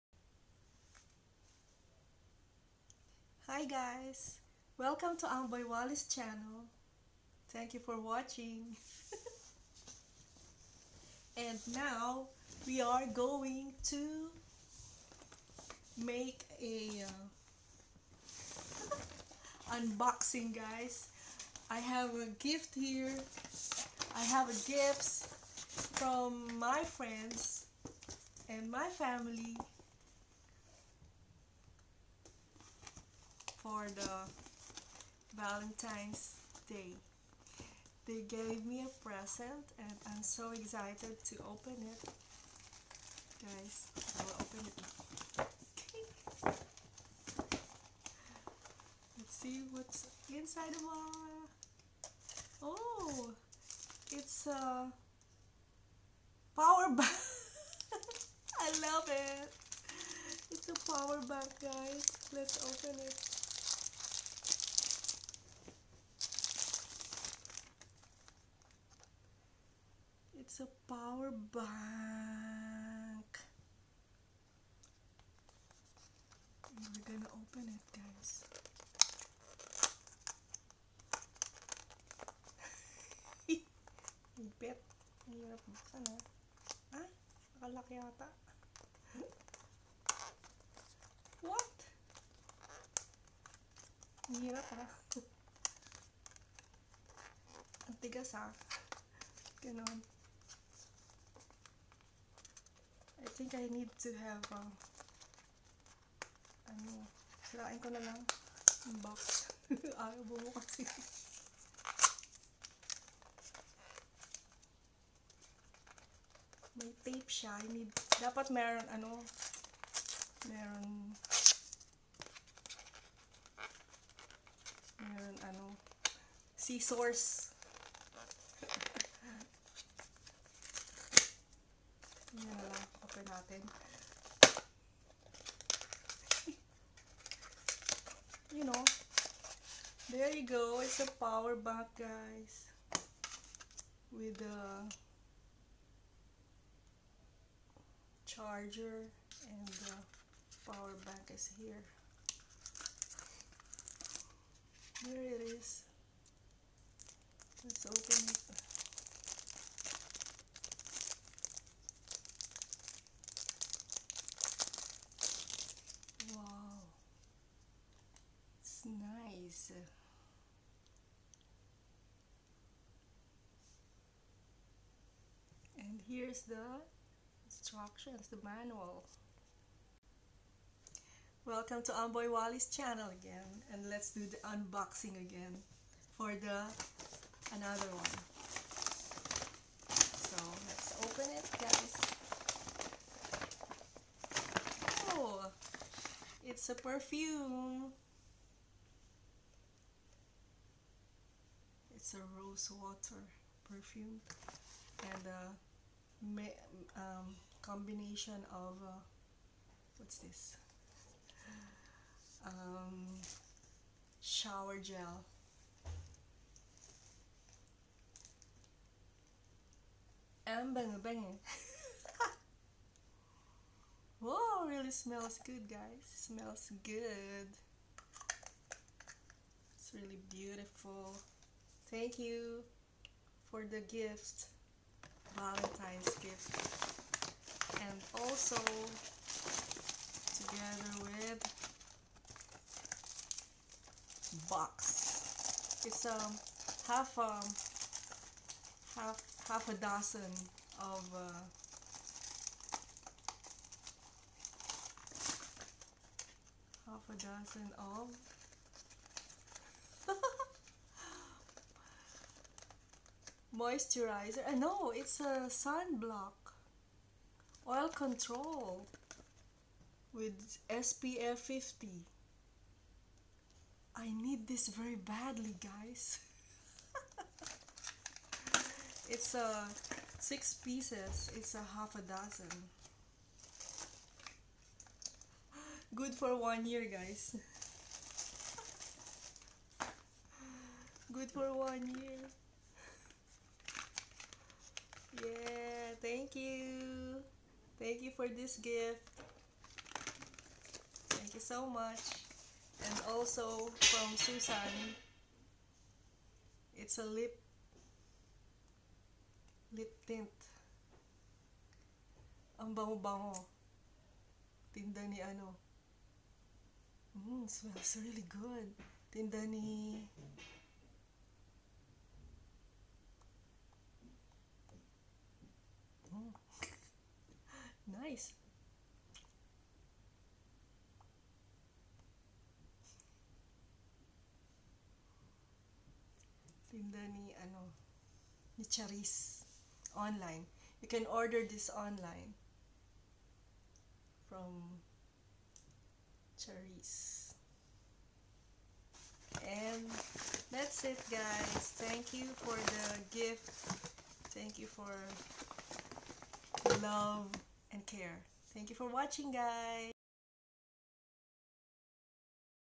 Valentine's day gifts Unboxing Late upload
Hey guys sobrang busy Kaya super late upload ng Unboxing na ito noong February 14 pa video ito Happy Valentine's everyday guys Love you all Enjoy watching and have fun

Thanks for the love and support mga ka Amboy dyan hehe